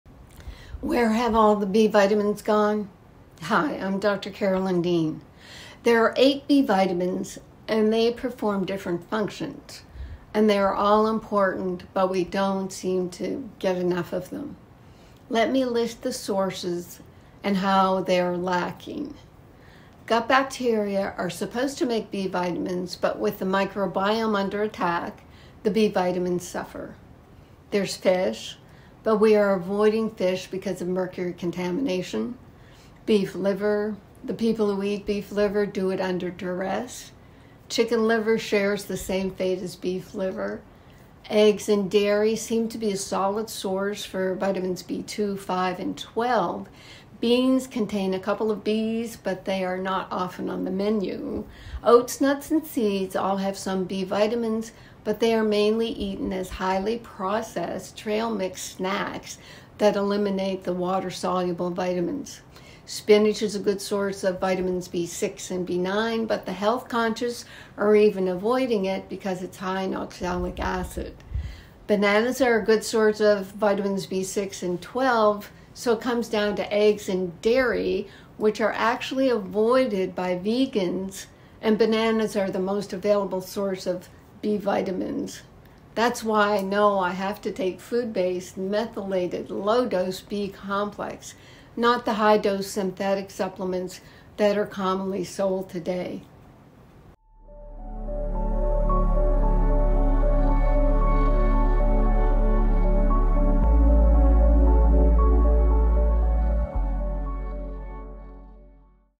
The Disappearance of B Vitamins with Dr. Carolyn Dean
🎙 Join Dr. Carolyn Dean, a distinguished health expert, in this video as she uncovers the mystery surrounding the scarcity of B vitamins in our diets. In today's fast-paced world, it's crucial to understand the sources of these essential vitamins and why they seem to be dwindling from our plates.

🌱 Dr. Dean discusses the significance of the eight B vitamins, each with its unique role in supporting our health. Despite their importance, many of us fall short in meeting our daily B vitamin requirements. Let's explore the sources and the reasons behind their inadequacy.

🦠 Our gut bacteria play a vital role in synthesizing B vitamins, but with the ongoing challenges to our microbiome, the production of these essential nutrients is compromised.

🐟 Fish, a traditional source of B vitamins, is often avoided due to concerns about mercury contamination.

🥩 Beef liver, another source, is not commonly embraced by those who consume it.

🥚 Eggs and dairy provide B vitamins, but these sources may be limited or avoided altogether, especially by vegans.

🌱 Dr. Dean highlights how beans, oats, nuts, seeds, spinach, and bananas also contain B vitamins but are often consumed in processed forms that strip away these vital nutrients.

🍌 Bananas, rich in vitamins B6 and B12, stand out as one of the most accessible sources of B vitamins.

👉 Recognizing the challenges in obtaining sufficient B vitamins from our diets, Dr. Carolyn emphasizes the importance of opting for food-based, methylated, low-dose B complex supplements as a practical solution. These supplements offer a convenient way to bridge the gap in our B vitamin intake, especially when natural sources may be limited or avoided.

Timestamps:
0:00 Intro
0:07 8 B Vitamins
0:20 Sources That Are Lacking B Vitamins
1:27 B Vitamin Supplements
1:38 End Credits

Discover the truth about B vitamins and how you can ensure your body gets the essential nutrients it needs.